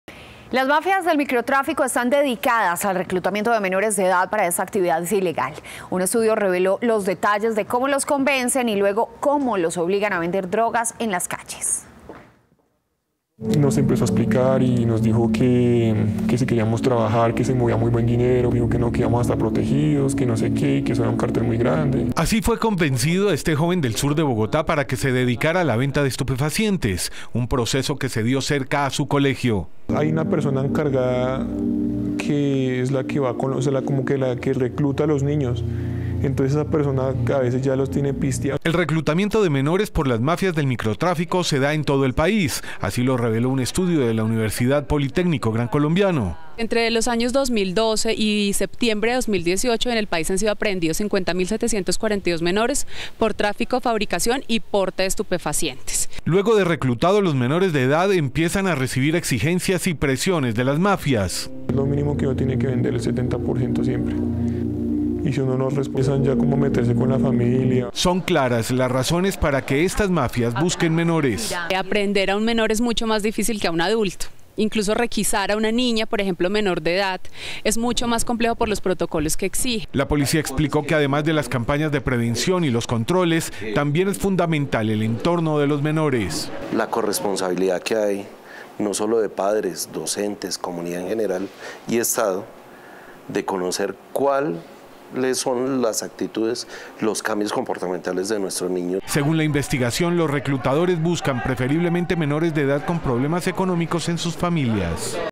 Crudo relato de un joven reclutado por narcos en Bogotá | Noticias Caracol
El adolescente cuenta cómo mafias los convencen de hacer parte del negocio y luego los obligan a distribuir droga en las calles.